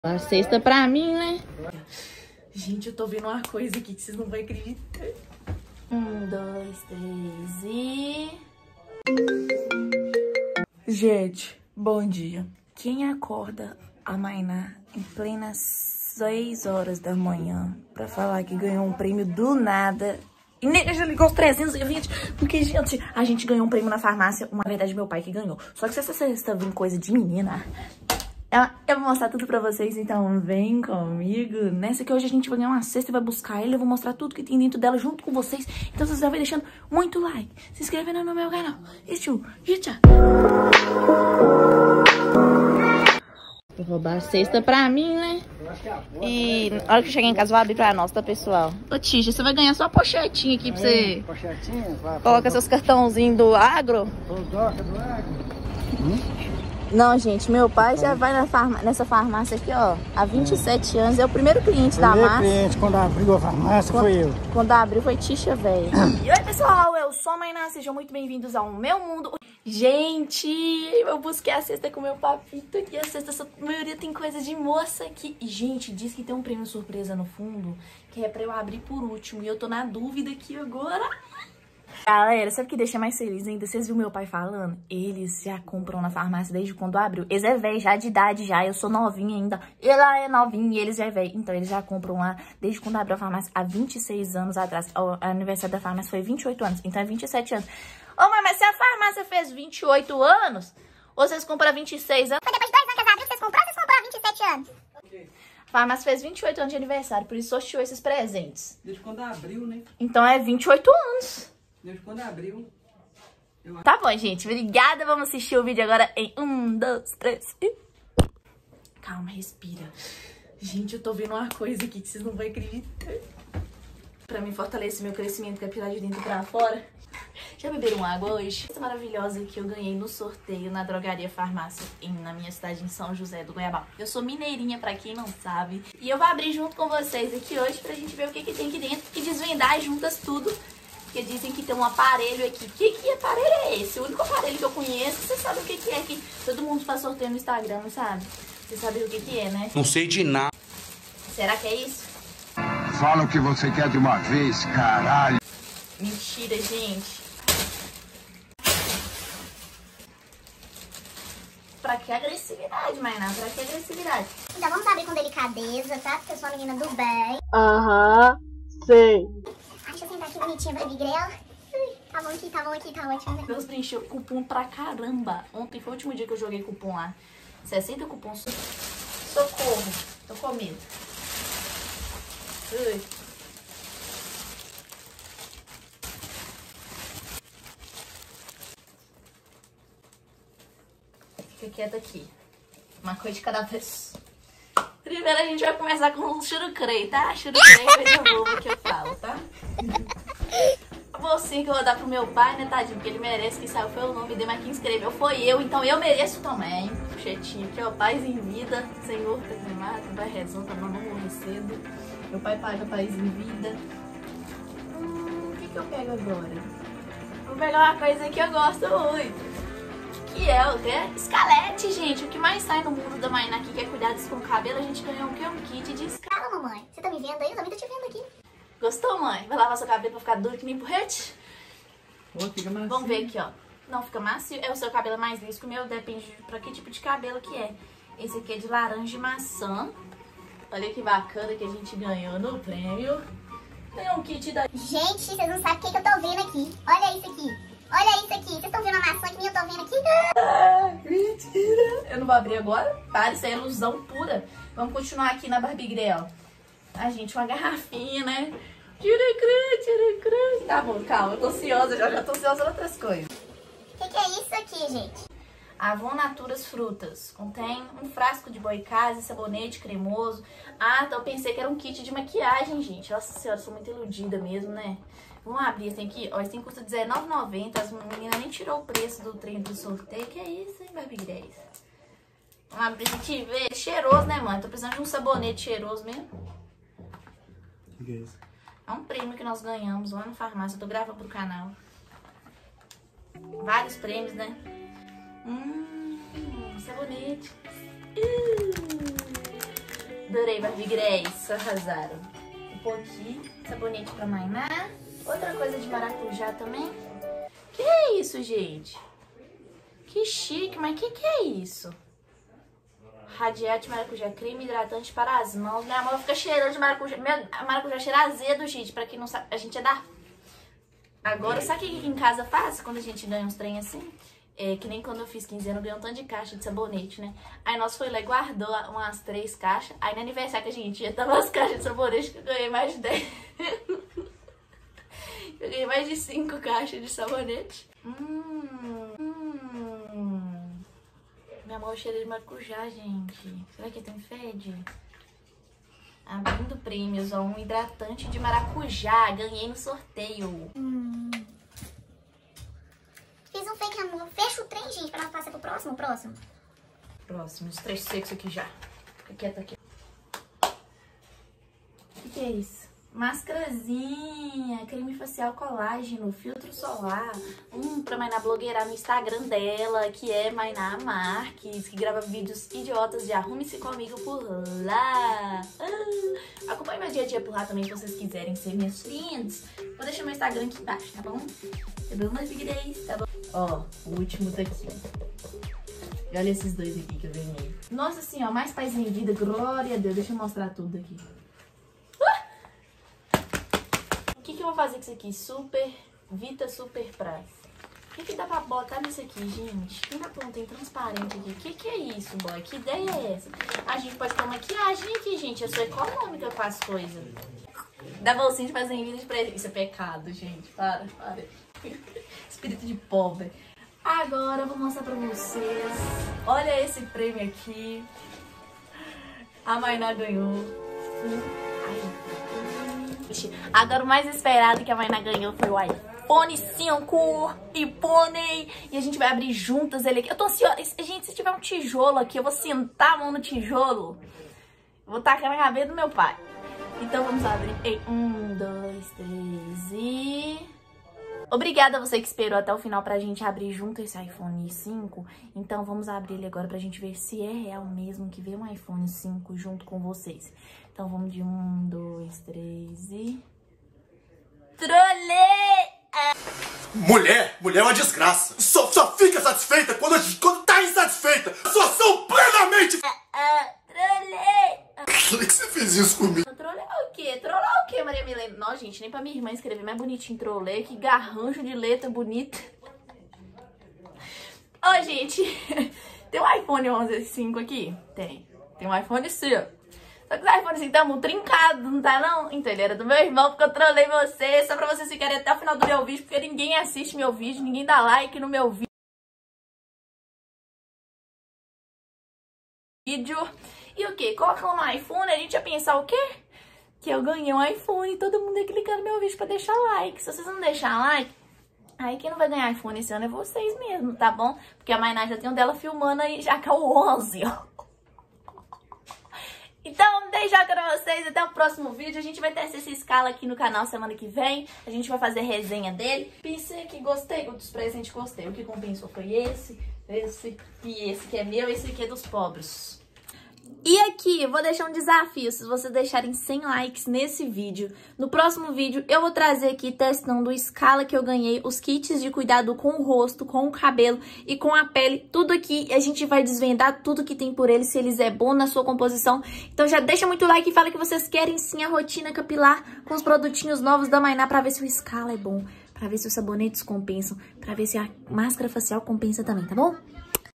GANHEI UM PRÊMIO NA MINHA CIDADE PELA 1° VEZ uma cesta + 1 iPhone 😍
Oii, barbigrels da Mainá GANHEI UM PRÊMIO NA MINHA CIDADE PELA 1° VEZ 🤧🥰🎁!!!
E nesse vídeo vou mostrar para vocês os mimos que ganhei dentro da cesta!! Veio muitas coisas de beleza que a Mainá vai testar aqui no canal se vocês assistir até o final e comprir com o combinado. Fechou? Hihi🥰💜 solta o playyyy